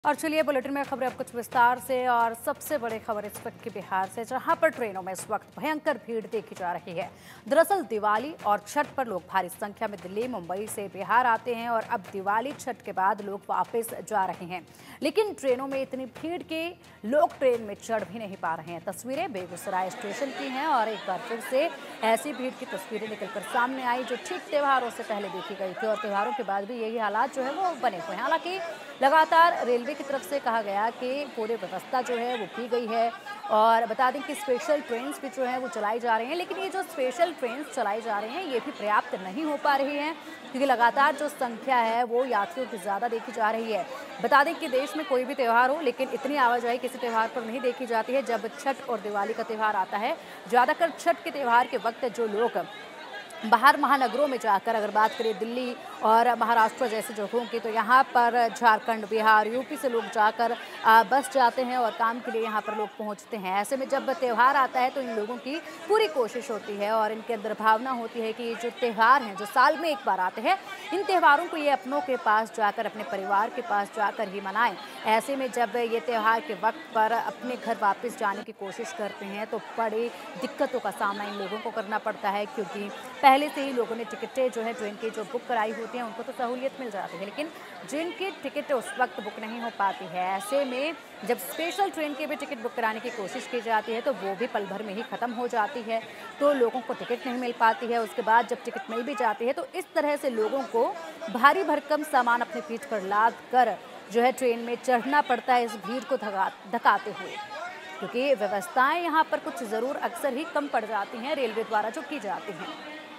Breaking News : आने के लिए मारामारी, जाने के लिए भी मारामारी! | Train | India News
त्योहारों के दौरान खुशनुमा पल बिताकर लोग अपने घरों से वापस बड़े शहर लौट जाएंगे। यह तो हर कोई जानता ही था। लेकिन ट्रेनों के जनरल कोच का जो हाल, त्योहार के दौरान था। वह त्योहार के बाद भी बदस्तूर वैसा ही बना हुआ है।

 Everyone knew that people will return to the big cities from their homes after spending happy moments during the festivals. But the condition of the general coaches of trains was the same during the festival. It remains the same even after the festival.